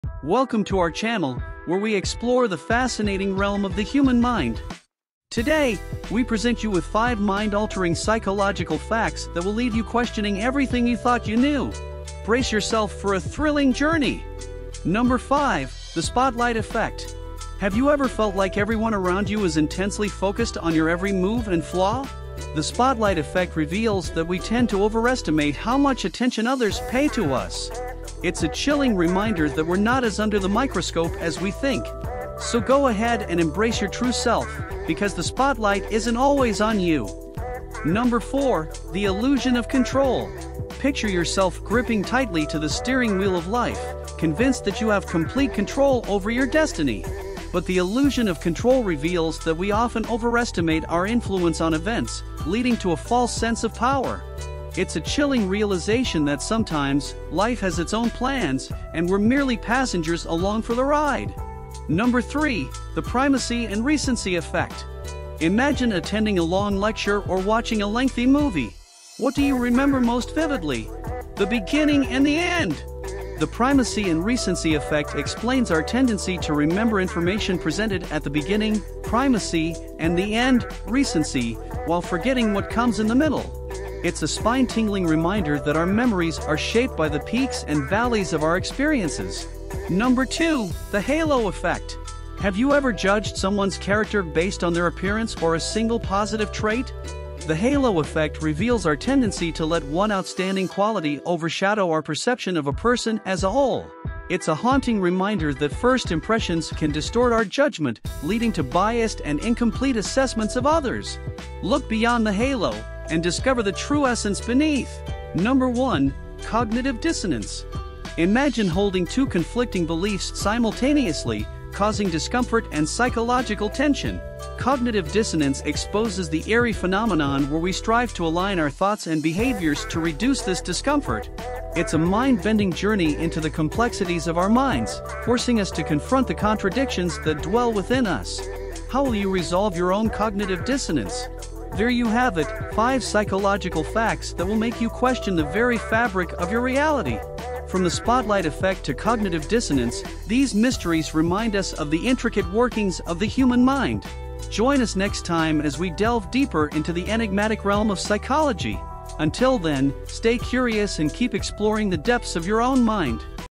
The Enigma Within: Unveiling 5 Mind-Altering Psychological Facts That Will Haunt You
In this thrilling video, prepare to have your mind twisted and your perception of reality shattered. Join us as we delve into the depths of the human psyche, revealing five mind-altering psychological facts that will leave you questioning everything you thought you knew. From the eerie Halo Effect to the chilling Cognitive Dissonance, these psychological enigmas will challenge your understanding of yourself and those around you. Brace yourself for a rollercoaster ride through the complexities of the mind, as we unravel the mysteries that lurk within. Get ready to be haunted by the shocking revelations that lie in wait. Are you prepared to confront the enigma within?